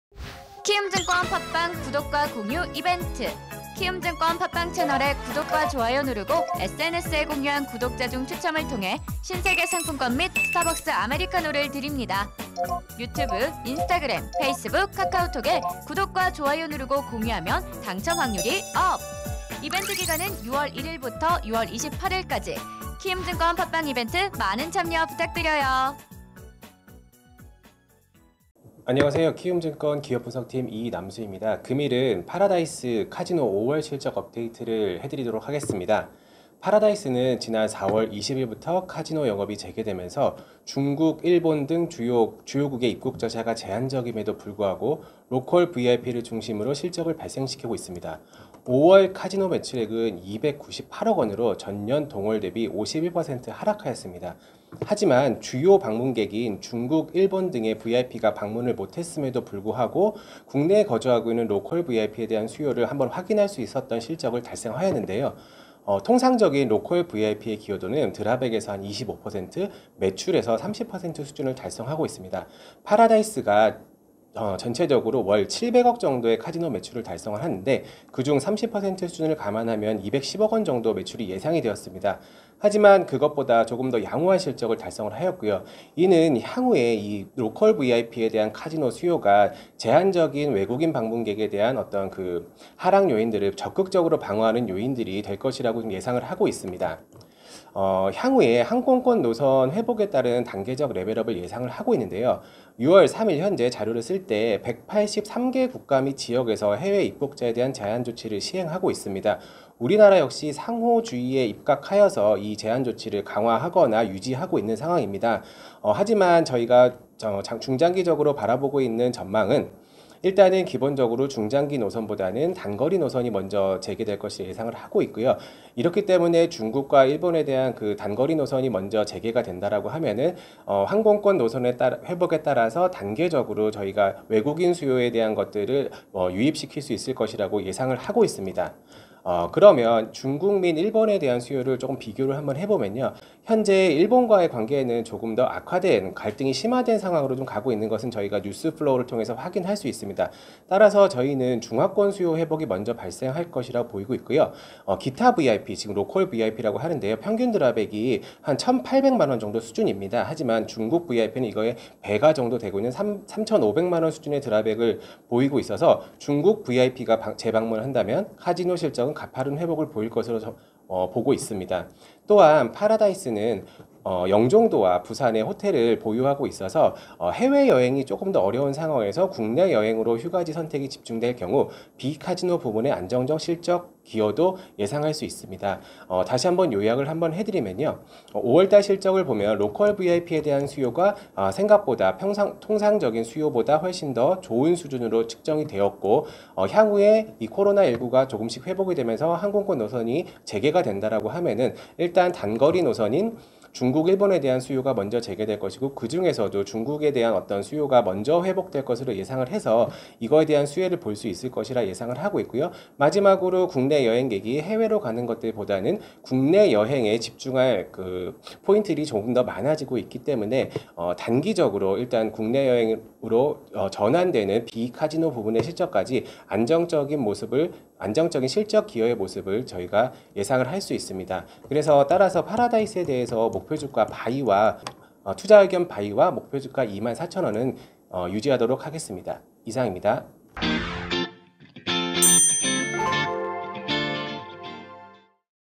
파라다이스, 항공권 노선 회복에 따른 단계적 레벨업 예상 (애널리스트 리포트.기업분석/20.06.08)
20일부터 카지노 영업이 재개되어 중국, 일본 등
주요국의 입국절차가 제 한적임에도 불구하고
로컬VIP를 중심으로 실적을 발생
로컬 VIP의 접근성이 우수한 서울과 영종도를 중심으로 양
호한 방문객과 드랍액이 기록되어 향후 항공권 노선 재개 및
입국절차 완화에 따른 실적 개선이 두드러 질 것으로 판단
기타VIP의 1인 평균 드랍액이 18백만원임에 반해,
1인 평균 35백만원 수준의 드랍액을 보이는 중국VIP가 재방문한다면
카지노 실적은 가파른 회복 을 보일 것으로 전망
이남수 연구위원 키움증권 기업분석팀